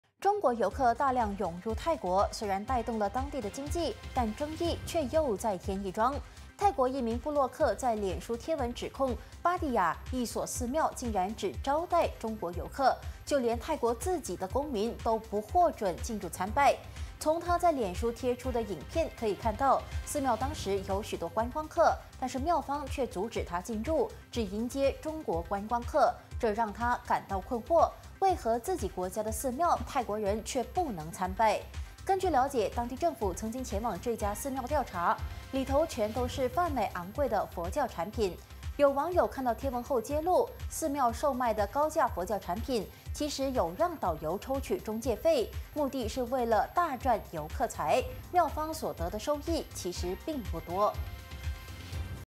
“只接中国客” 泰国寺庙拒本国公民入内惹议
中国游客大量涌入泰国，虽然带动了当地经济，但争议却又再添一桩。